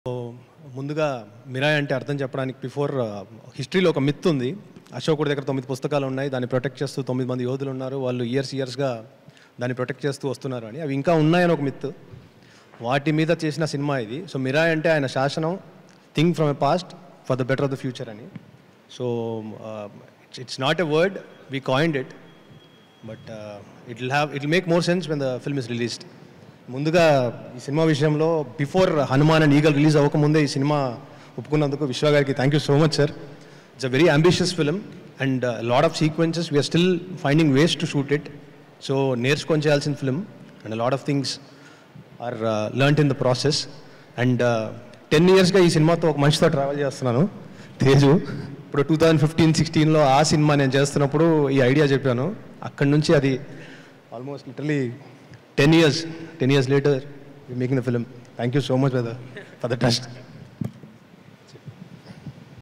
Director Karthik Ghattamaneni reveals the Story & Meaning of MIRAI @ Mirai Launch Event | Gulte.com
WATCH: Director Karthik Ghattamaneni reveals the Story & Meaning of MIRAI @ Mirai Launch Event | Gulte.com